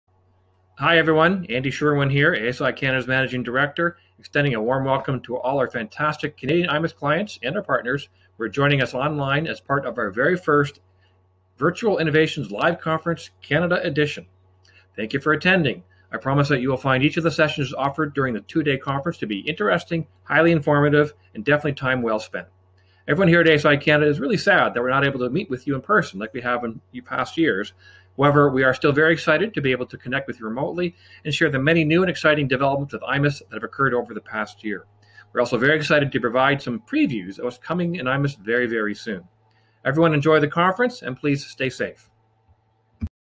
Andrew Sherwin - ASI
Hear a short statement from Andrew Sherwin of ASI on iNNOVATIONS LIVE!